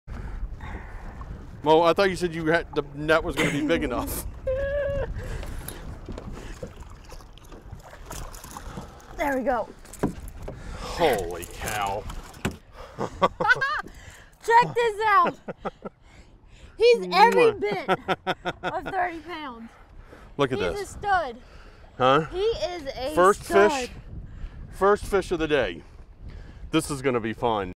Louisiana Redfish with Moe Newman Preview
Moe Newman and Carter Andrews are having a great time fishing for 30 lb bull redfish (red drum) in Venice, Louisiana!